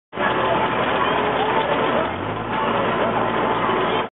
N & M bravery!
N&M  go up the Ozone Drop!  They prove their mettle when it comes to park rides!